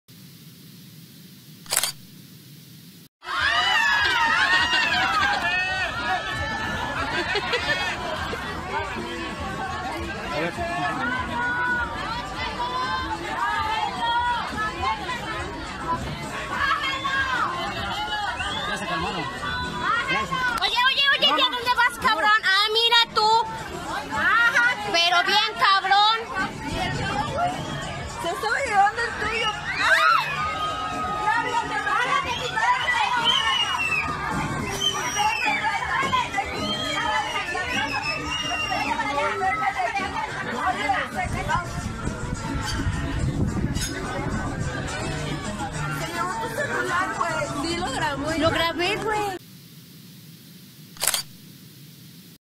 Crook Nicks Phone From Woman Distracted By Boat Bust-Up
This is the moment an alleged thief appears to take a mobile phone from a woman's bag as she is distracted by a fight between two men on a colourful canal boat.

The incident occurred in Xochimilco, a neighbourhood in the south of the Mexican capital of Mexico City and was recorded by a woman who was filming the fight.

The footage shows two men, reportedly drunk, being held back from fighting on a trajinera (a traditional Mexican boat from the region) as some onlookers scream and others laugh.

A man wearing a black polo shirt then walks in front of the camera and puts his hand into a bag placed on the table in front of the camera, touching a mobile phone but not stealing it.

Seconds later he returns and tries to steal the device, however, the woman, who continued recording, notices the attempt and confronts the stranger along with other women who were also on the boat.

Two women angrily demand that he changes to another boat and warn the other people on the other boat to watch their belongings.

Social media users shared the video to warn people of the thief's technique of taking advantage of distracted tourists to steal valuables.

The neighbourhood of Xochimilco is a tourist hot spot in Mexico City due to the last remains of a huge system to transport water that the Aztecs built and its dense vegetation.